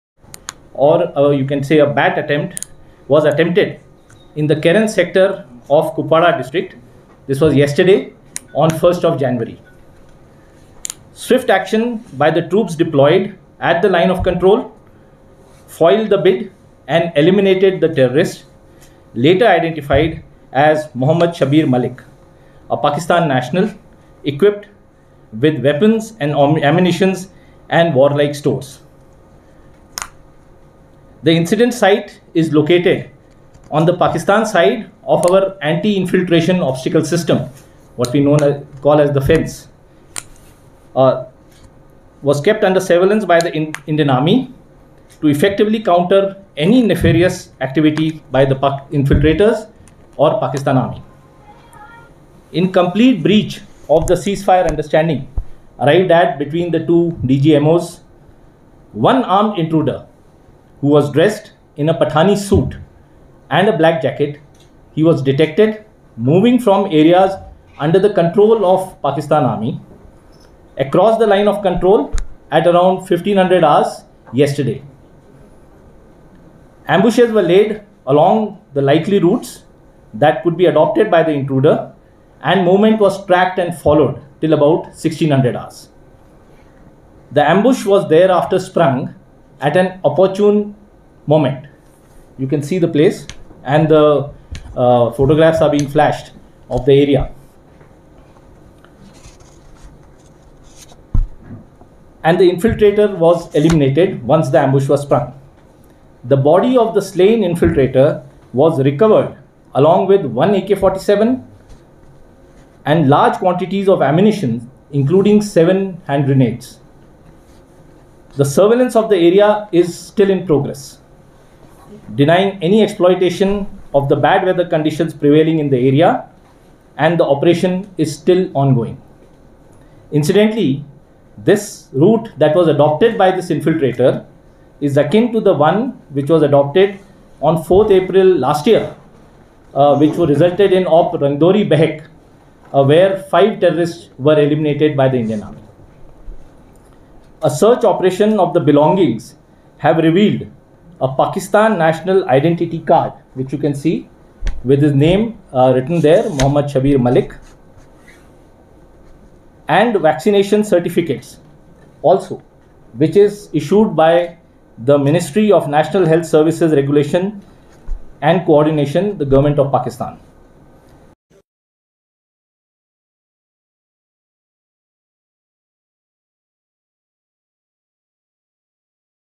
INFILTRATION BID ELIMINATED IN KEREN SECTOR OF KUPWARA ON 01 JAN 2022
Srinagar, 02 Jan 2022. In a complete breach of the ongoing Ceasefire Understanding between the two armies across the line of control, an inflitration or BAT action was attempted in the Keran Sector of Kupwara district on 01 Jan. Swift action by the troops deployed at the Line of Control foiled the bid and eliminated the terrorist, later identified as Mohd Shabbir Malik, a Pakistani national, equipped with weapons, ammunition and warlike stores. The incident site is located on the Pakistani side of the Anti Infiltration Obstacle System, kept under surveillance by the Indian Army to effectively counter any nefarious activity by the infiltrators or Pakistan Army.

In complete breach of the Ceasefire Understanding arrived at between the DGMO's, one armed intruder dressed in Pathani suit and black jacket was detected moving from areas under the control of Pakistan Army across the Line of Control at around 1500 hours on 01 Jan 21. Ambushes were laid along likely routes that could be adopted by the intruder and movement was followed  till 1600 hours. The ambush was sprung at the opportune moment and the infiltrator eliminated. The body of the slain infiltrator was recovered along with one AK 47 and large quantity of ammunition including seven grenades. The surveillance of the area is in progress, denying any exploitation of the bad weather condition prevailing in the area and the operation is ongoing. The route adopted by the terrorist was similar to the one adopted on 04 Apr 20 during Op Rangadori Bhaikh in which five terrorists were eliminated.

A search of the belongings revealed Pakistani National  identity card and vaccination certificates (issued by Ministry of National Health Services Regulation & Coordination Government of Pakistan)  identifying him as Mohd Shabir Malik. The belongings also include a photo of the infiltrator in Army Uniform wearing name tab of Shabbir.

This clearly establishes that Pakistan continues to sponsor cross border terror.  A Hotline communication has been made to Pakistan Army asking them to take back the body of the killed individual.